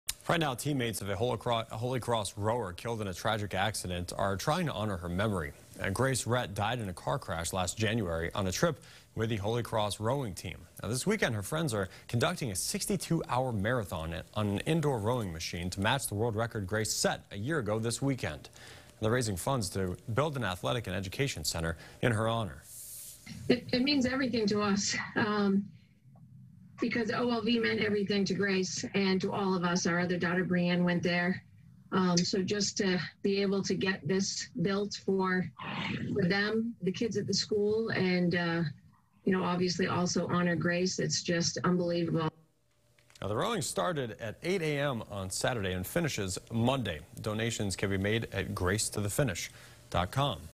62-Hour Rowing Marathon Held To Honor Holy Cross Rower Grace Rett
Grace Rett was killed in a car crash in Florida with the rowing team last January.